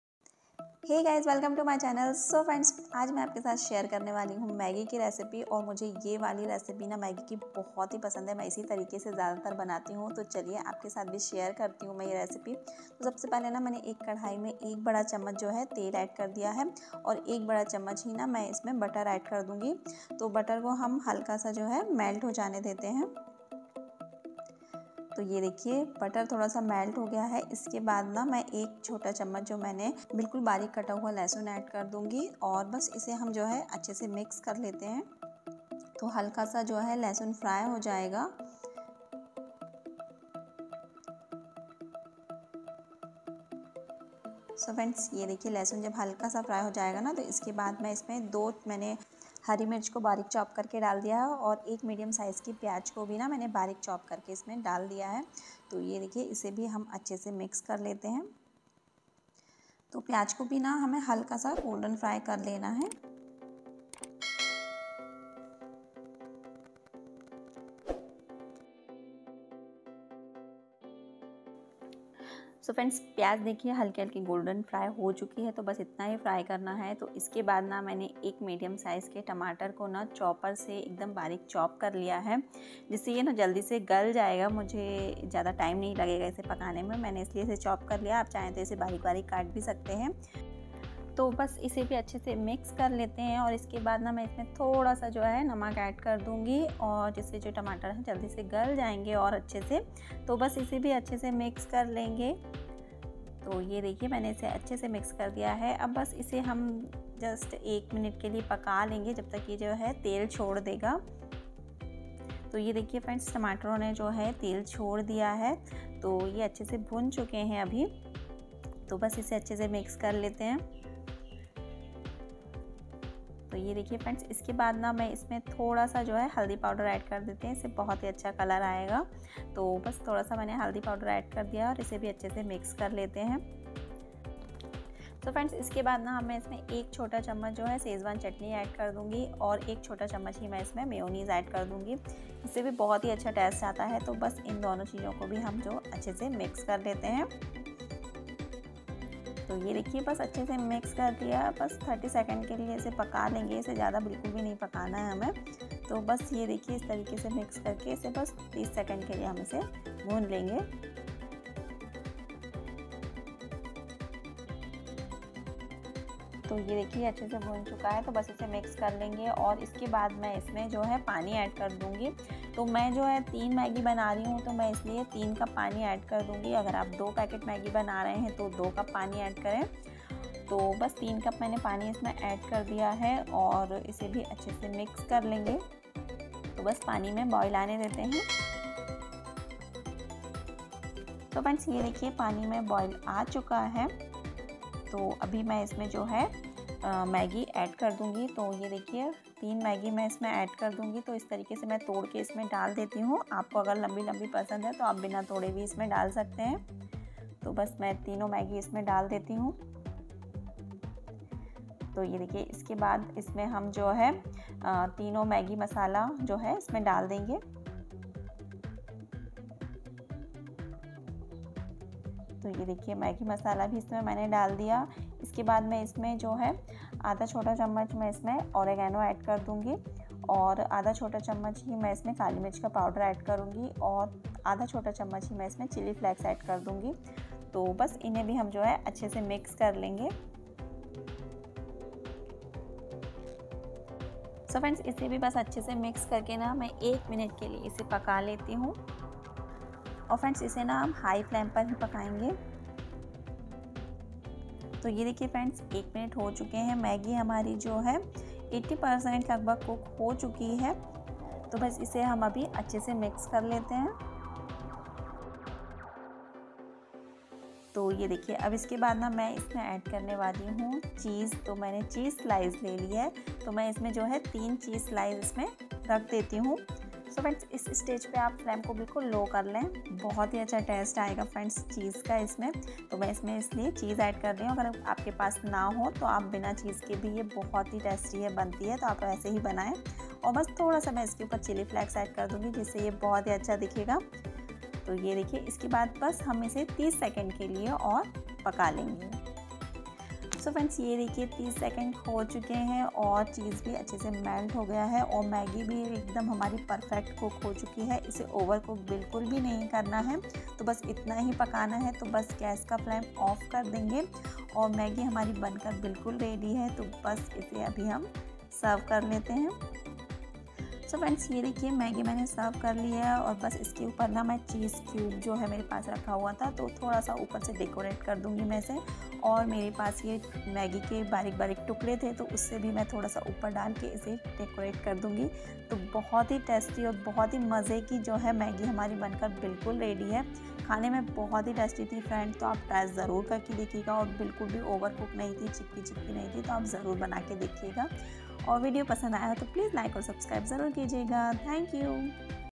Perfect Maggi Masala Recipe 😋 | Chatpati Masaledar Maggi Banane Ka Tarika | Cheezy Maggi Recipe 😍
Perfect Maggi Masala Recipe 😋 | Chatpati Masaledar Maggi Banane Ka Tarika | Best Maggi Recipe 😍 |

Ingredients:
Maggi - 3 packets
Fine chopped garlic - 1 tsp
Green chilli - 2
Fine chopped onion - 1 medium size
Fine chopped tomato - 1 medium size
Salt - 2 pinch
Turmeric powder - a little bit
Schezwan chutney - 1 tsp